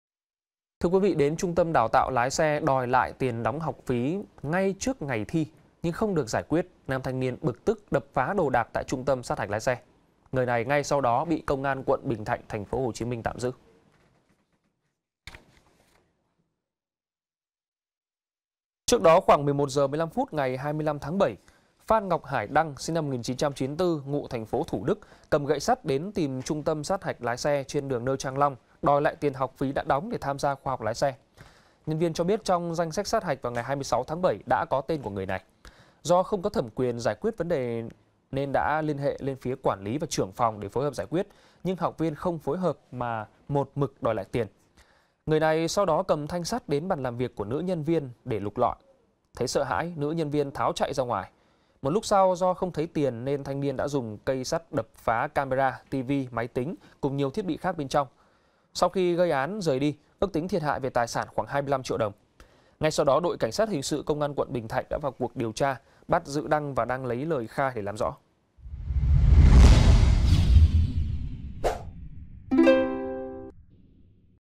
Đập phá Trung Tâm Sát Hạch Lái Xe vì không đòi được Tiền Học Phí| VTC14
VTC14 |Đến trung tâm đào tạo lái xe đòi lại tiền đóng học phí ngay trước ngày thi nhưng không được giải quyết, nam thanh niên bực tức đập phá đồ đạc tại trung tâm sát hạch lái xe. Người này ngay sau đó đã bị Công an quận Bình Thạnh, TP.HCM tạm giữ.